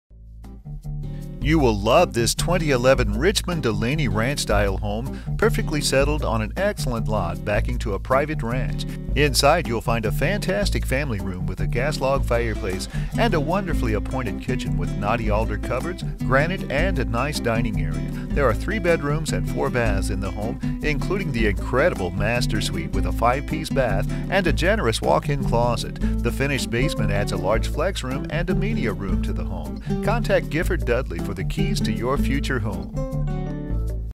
Home for sale in CASTLE ROCK, CO | $465,000
Why Buy New?? Landscaping and Blinds Are Done For You!!! You Will Love This 2011 Richmond Delaney Ranch Style Home.  This Home Sits On An Excellent Lot, Backing A Private Ranch, Enjoy The Scrub Oak, Rolling Hills and No Homes Directly Behind.  This 3 Bedroom, 4 Bathroom Home Is Move in Ready, The Great Room Is Fantastic, Gourmet Kitchen Is Open To The Family Room With Gas Log Fireplace, Kitchen Is Wonderfully Appointed With Yards Of Granite Countertops, Ample Knotty Alder Cupboards, Access To The Professionally Designed Outdoor Living Space With The View That Is Surreal.  The Master Bedroom Is On The Back Of The Home, 5 Pc Bath With Generous Walk in Closet, Secondary Bedroom Is Good Sized and The Main Level Includes Powder Room, Formal Dining, Main Floor Study and Laundry Room.  The Builder Finished The Basement Which Adds Another Bedroom, A Large Flex Room, Game/Media Room Area, Three Quarter Bath and Storage! Southern Exposure Melts The Snow!!!